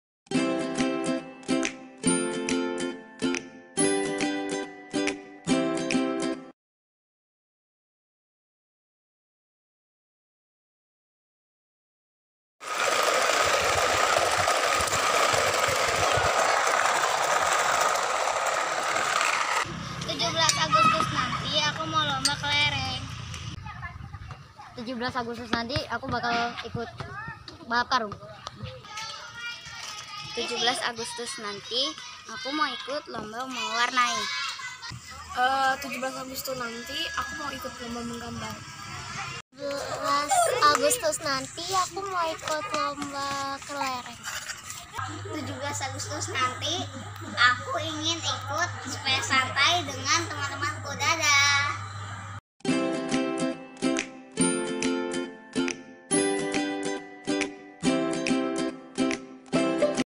cerita 17 agustus nanti ? begini kalo anak anak bicara soal kemerdekaan Indonesia yang diisi lomba
expresi anak anak saat ditanya mau ikutan lomba agustusan nanti